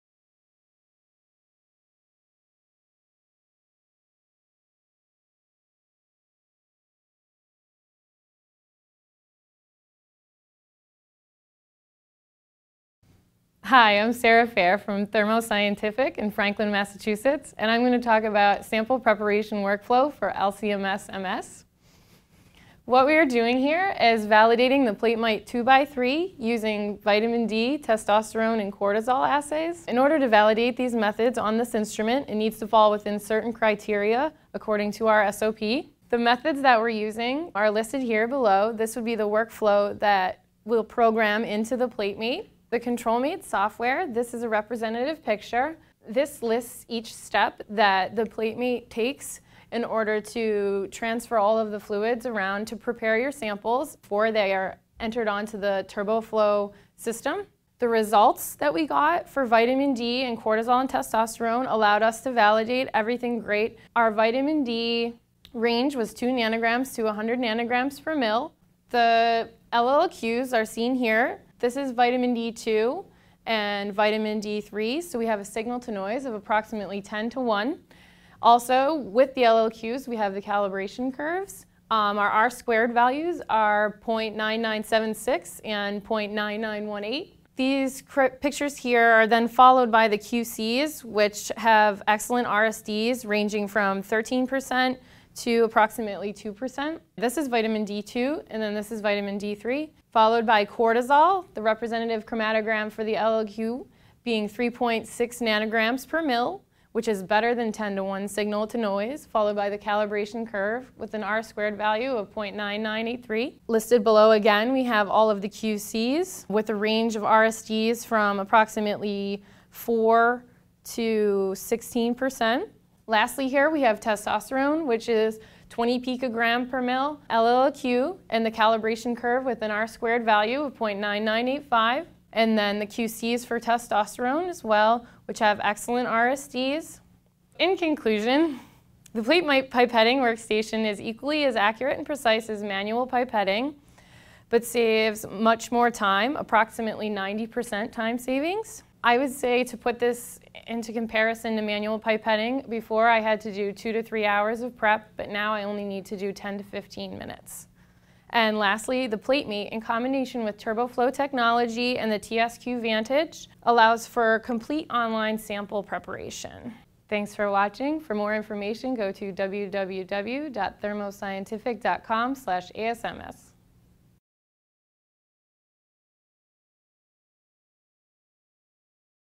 Increasing Speed of UHPLC-MS Analysis of Vitamin D for Research Use
Sarah Fair presents her scientific poster about removing manual pipetting from the analysis of vitamin D metabolites, testosterone and cortisol using the accuracy and precision of the Thermo Scientific Platemate Pipetting Workstation. Using the automated system offered significant time savings and passed all acceptance criteria for the compounds tested.